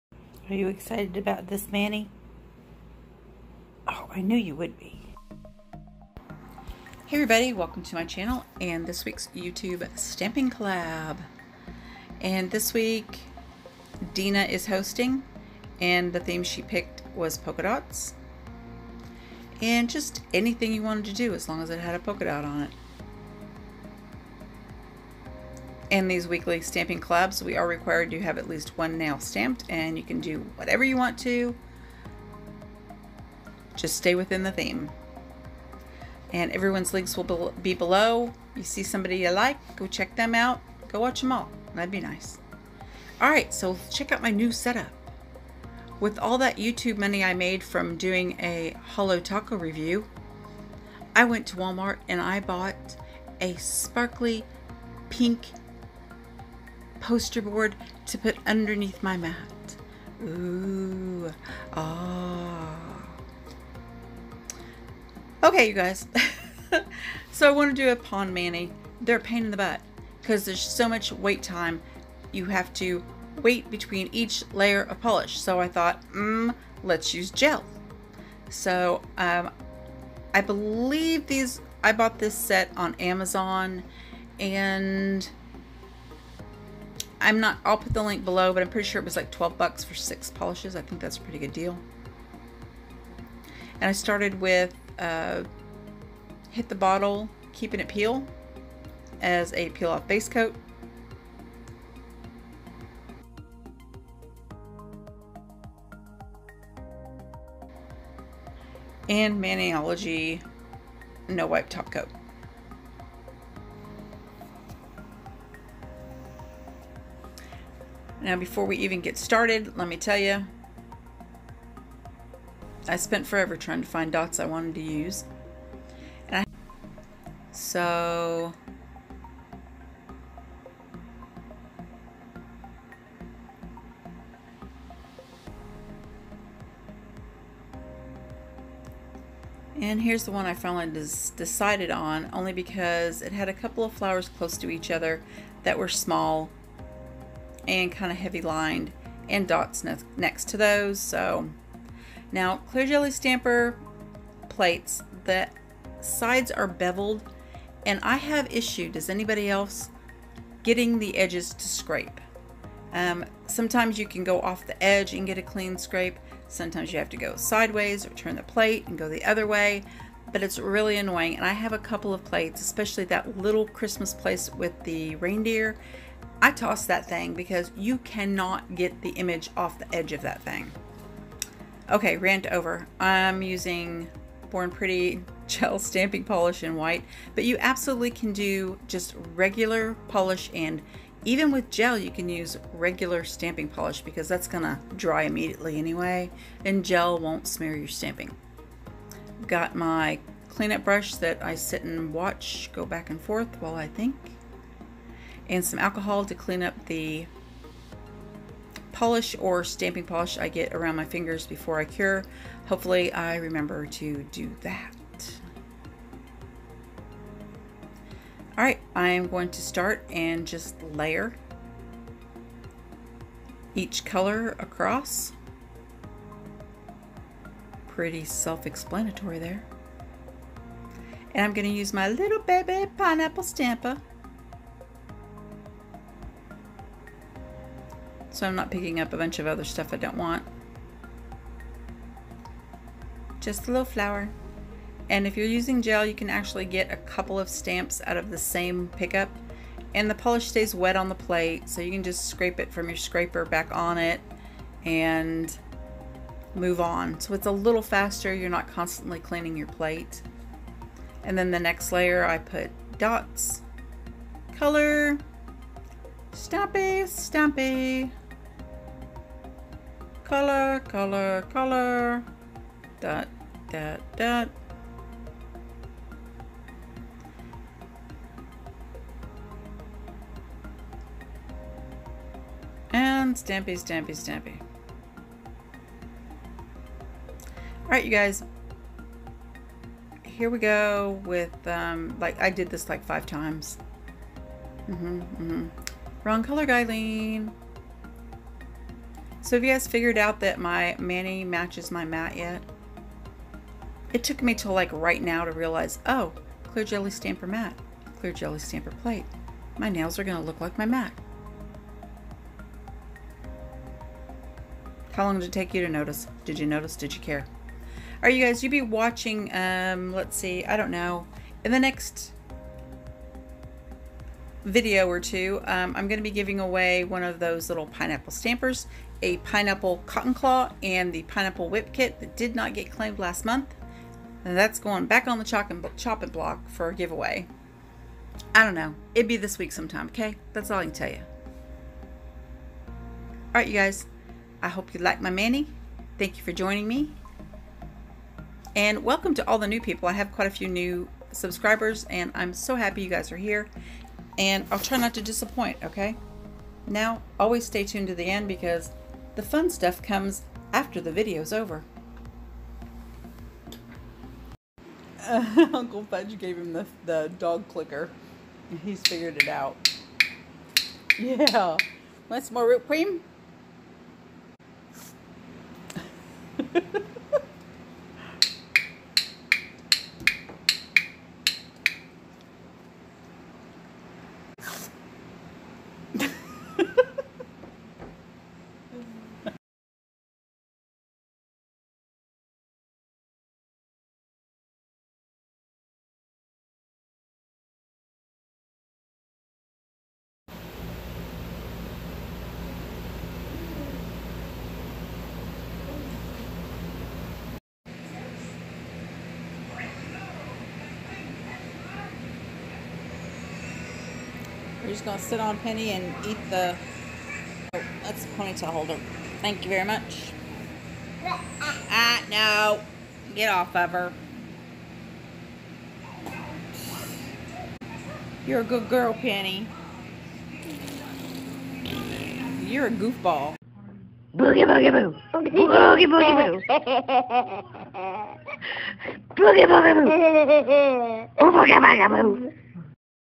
Pond Mani with Gel, no waiting around for it to dry!
This weeks Youtubers weekly stamping collab is host by Dina, she picked Dots.  So I did a dotted and flowered Pond mani.
I have linked all the ladies channels below and all the links to the products I used.

Product list:
Beetles Jelly Gel polish

Born Pretty Gel stamping polish

Clear Jelly stamper plates  I buy from Lantern and wren because the shipping from Canada is too high. So if you are in Canada, order from the CJS site.  Google it yourself.

📌Maniology link use.   GUYLENE325

📌10% on any Beautybig bang use code
GL325

Nature+5 LED UV Lamp

My watermarbling hat pins

Ebasket foil wraps